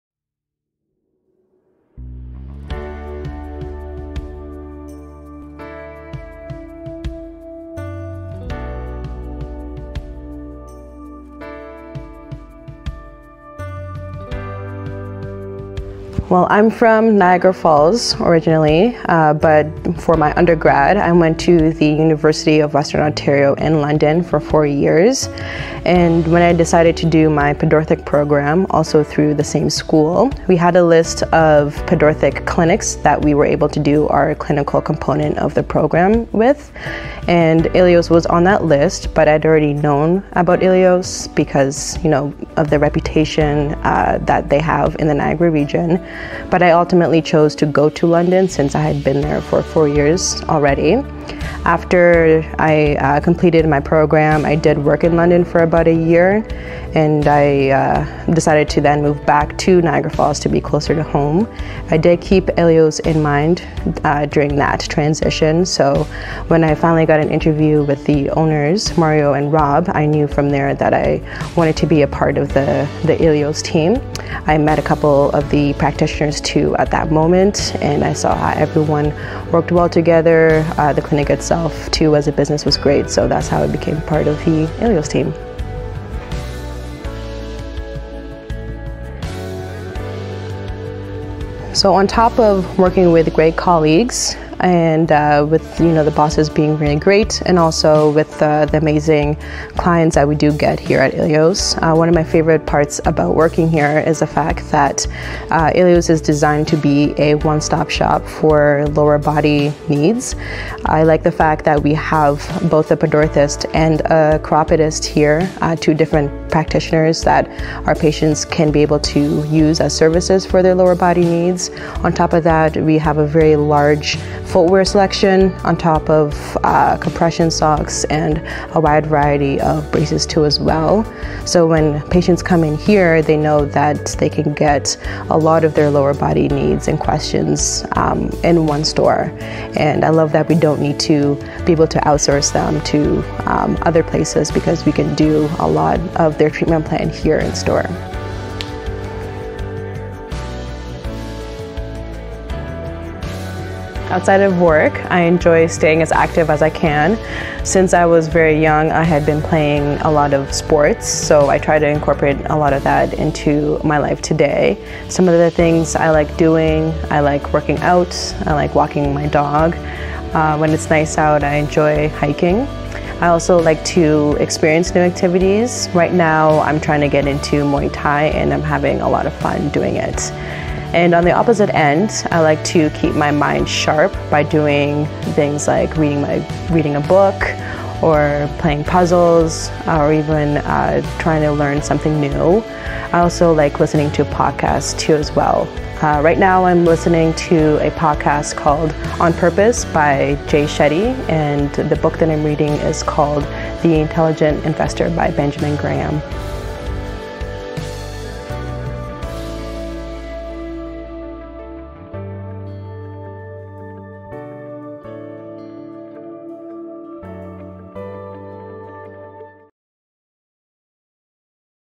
Ashley Njeru
Meet Ashley Njeru, Canadian Certified Pedorthist at Elio's Foot Comfort Centre